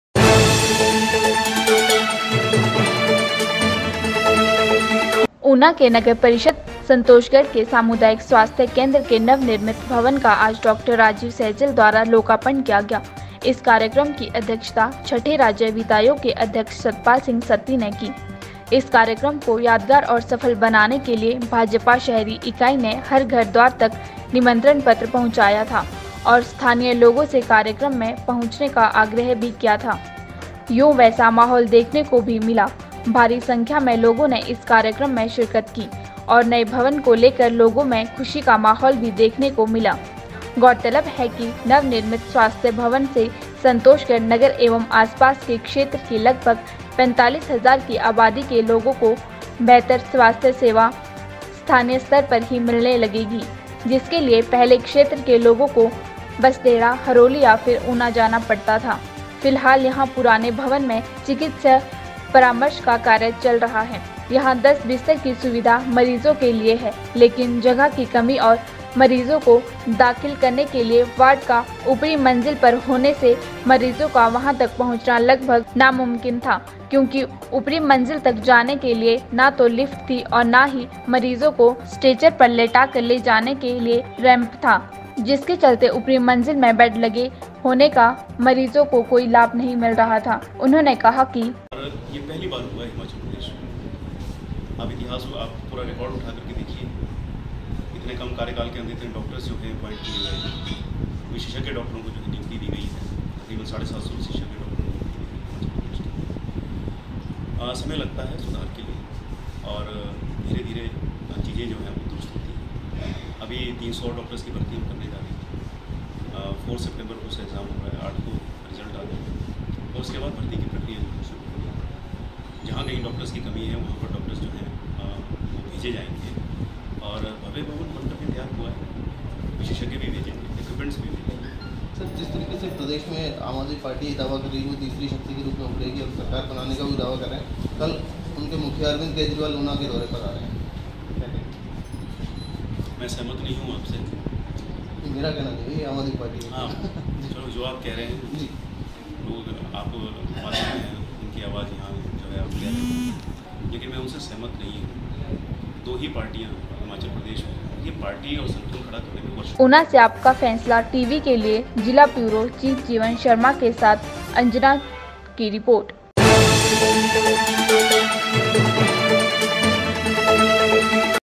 ऊना:-सामुदायिक स्वास्थ्य केंद्र के नवनिर्मित भवन का डॉ.राजीव सहजल द्वारा लोकार्पण-----
ऊना के नगर परिषद संतोषगढ़ के सामुदायिक स्वास्थ्य केंद्र के नवनिर्मित भवन का आज डॉ.राजीव सहजल (स्वास्थ्य एवं परिवार कल्याण मंत्री हिमाचल सरकार) द्वारा लोकार्पण किया गया । इस कार्यक्रम की अध्यक्षता छठे राज्य वितायोग के अध्यक्ष सतपाल सिंह सत्ती ने की। आप को बता दे कि
इस कार्यक्रम को यादगार और सफल बनाने के लिए भाजपा शेहरी इकाई ने हर घर द्वार तक निमंत्रण पत्र पहुँचाया था और स्थानीय लोगो से कार्यक्रम में पहुँचने का आग्रय भी किया था, योय वैसा माहौल देखने को भी मिला, भारी संख्या में लोगो ने इस कार्यक्रम में शिरकत की ओर नए भवन को लेकर लोगो मे खुशी का माहौल भी देखने को मिला।
गौरतलब है कि
नवनिर्मित सवास्थ्य भवन से संतोषगढ़ नगर एवम आसपास के क्षेत्र की लगभग 45 हजार की आवादी के लोगो को वेहतर स्वास्थ्य सेवाएं स्थानीय स्तर पर ही मिलने लगेगी, जिसके लिए पहले क्षेत्र के लोगो को बसदेहड़ा, हरोली या फिर ऊना जाना पड़ता था।
फिलहाल यहां पुराने भवन में चिकित्सीय परामर्श का कार्य चल रहा है।  यहां 10 बिस्तर की सुविधा मरीजों के  लिए है, लेकिन जगह की कमी और मरीजो को दाखिल करने के लिए वार्ड का ऊपरी मंज़िल पर होने से मरीजों का वहां तक पहुंचना लगभग नामुमकिन  था। क्योंकि ऊपरी मंजिल तक जाने के लिए ना तो लिफ्ट थी और ना ही मरीज को स्टेचर पर लिटा कर ले जाने के लिए रैम्प था ।जिसके चलते ऊपरी मंजिल में बैड लगे होने का मरीजो को कोई लाभ नही मिल रहा था । लेकिन नए भवन के निर्माण में इन सभी बिंदुओं पर बारीकी से ध्यान रक्खा गया है ऊपरी मंजिल तक जाने के लिए वकायदा रैम्प बनाए गए है जिससे मरीज को स्टेचर पर लिटा कर कहीं भी ले जाया जा सकता है। इस नए भवन में सीएचसी शिफ्ट होते ही 30 बिस्तर की सुविधा मरीजों के लिए उपलब्ध हों गई है सबसे बड़ा लाभ गर्भवती महिलाओं को मिलेगा जिन्हें इमरजेंसी में प्रसव पीड़ा के दौरान सामान्य रूप से होने वाली डिलिवरी यहीं संभव हो सकेगी जोकि पुराने भवन में कम स्थान होने के चलते संभव नहीं थी।

नही बढ़ी चिकित्सकों की संख्या।

फिलहाल किसी नए चिकित्सक की जहाँ तैनाती होने की कोई सूचना नही मिली है जिसके चलते पहले की तरह तीन चिकित्सक ही कार्यभार सम्भालेगे।और क्षेत्र के लोगो की सेवा करेंगे।

इतना हुआ खर्च।

इस भवन के निर्माण पर सरकार की ओर से लगभग साढ़े चार करोड़ रुपये की राशि खर्च की गई है। हिमाचल प्रदेश लोक निर्माण विभाग की और से इस कार्य को निजी कम्पनी से करवाया गया जिसके लिए अनुमानित राशी लगभग 448.01 लाख रुपय कम्पनी को दी गई |
ऊना से आपका फैसला टीवी के लिए जिला ब्यूरो चीफ जीवन शर्मा के साथ अंजना की रिपोर्ट